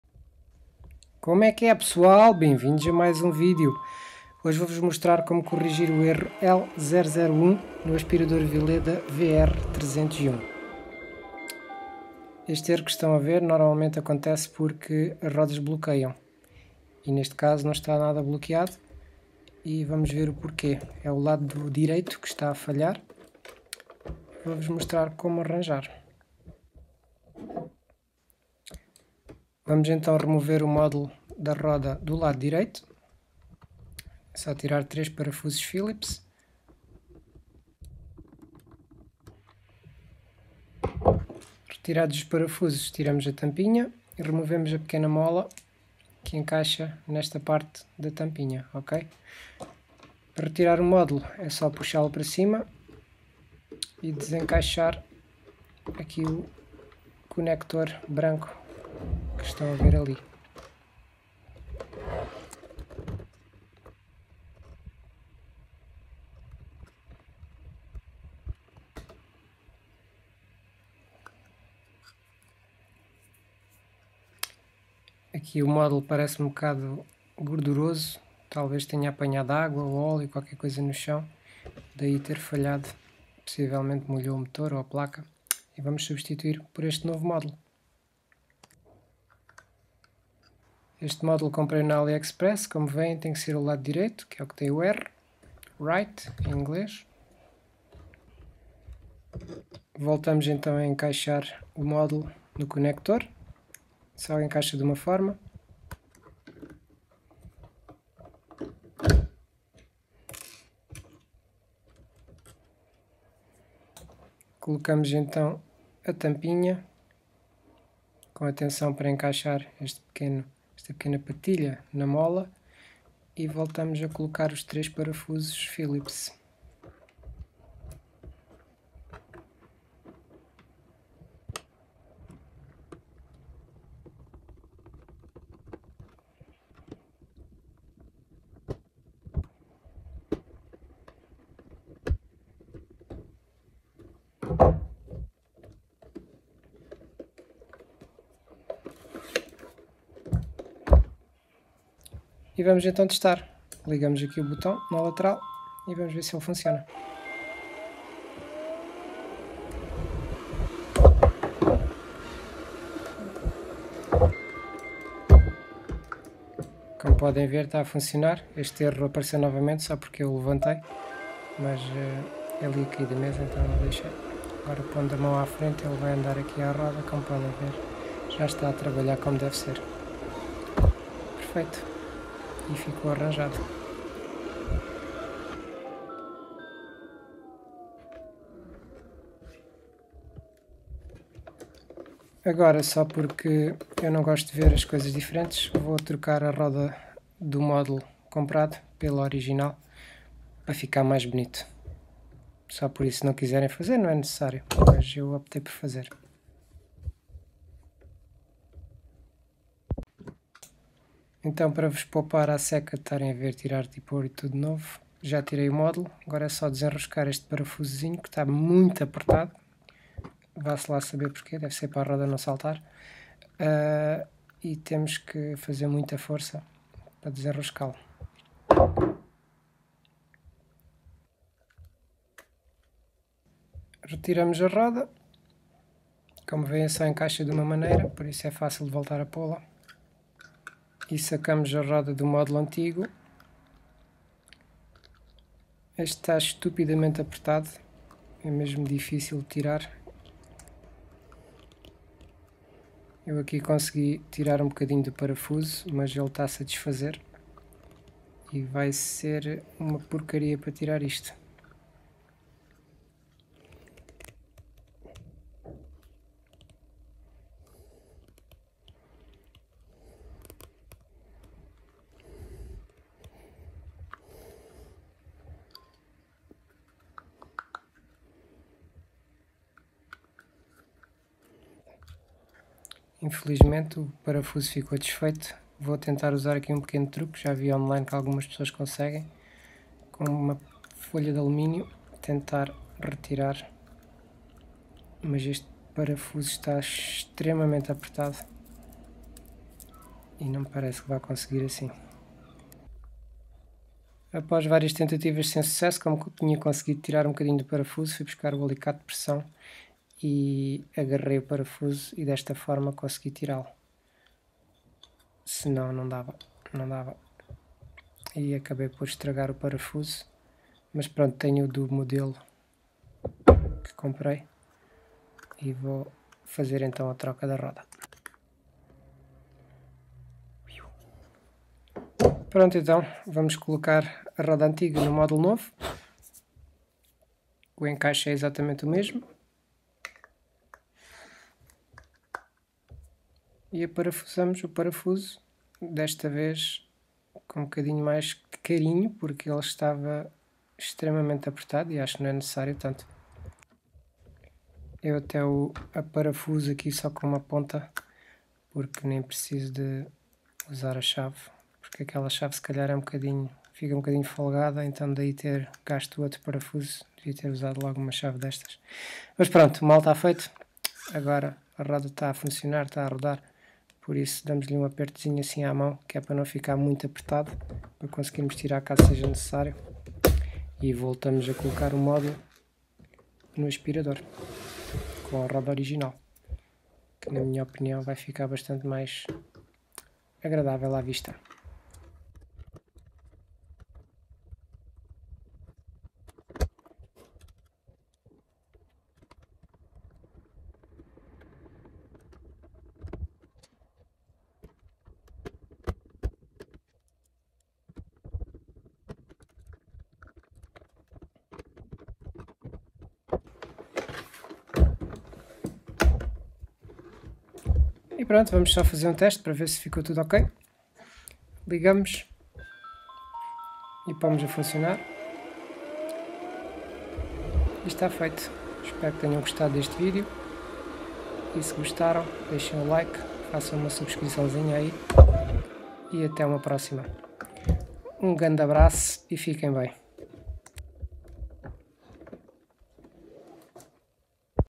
ASPIRADOR VILEDA VR 302 ARRANJAR O ERRO L001
Como reparar aspirador Vileda VR302  com erro L001
Modulo de roda do aliexpress.
Aspirador, IRobot, Roomba, Vileda.
ERROR E000 L001

Vileda VR 302 com rodas bloqueadas